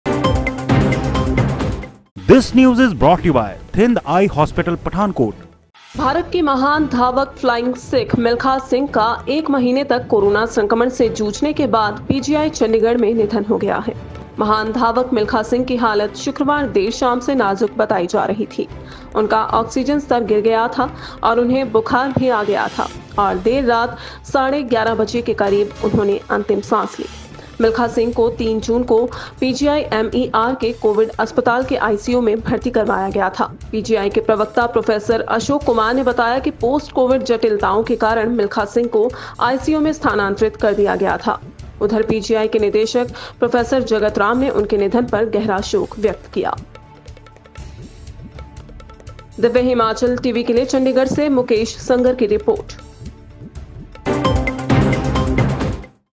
उड़न सिख मिल्खा सिंह का निधन, पीजीआई चंडीगढ़ में ली अंतिम सांस
divyahimachaltv
dhtv,tvnews
local news
divyahimachalgoodmorning
khabre ftafat
divyahimachalgoodafternoon